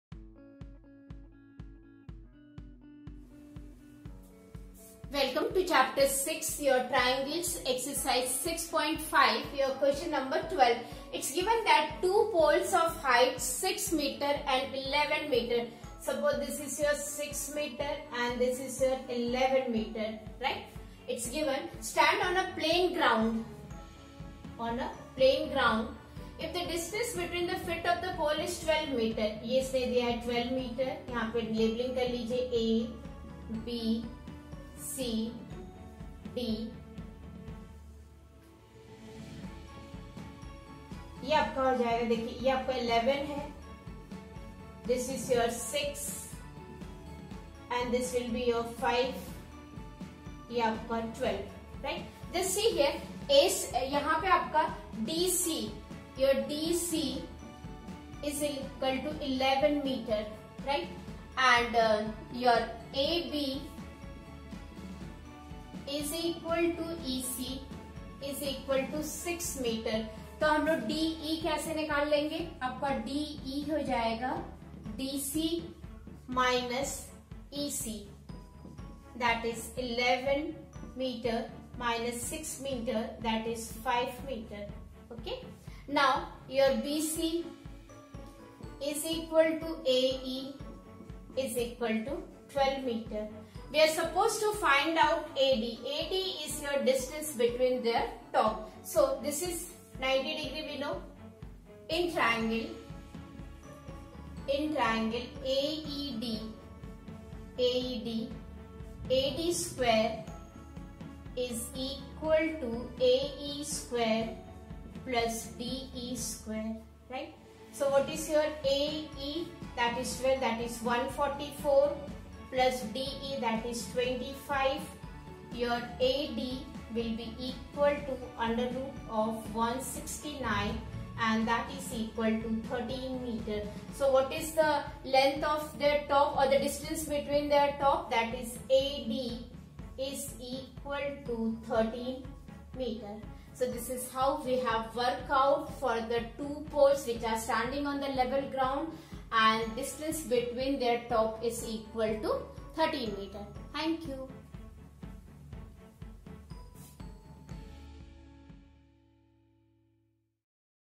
two poles of height 6m and 11m stand on a plane ground. if the distance between the feet ......
Class 10,
 Chapter Triangles,
Q.12)  Two poles of height 6m and 11m stand on a plane ground. if the distance between the feet of the pole is 12m . Find the distance between their tops?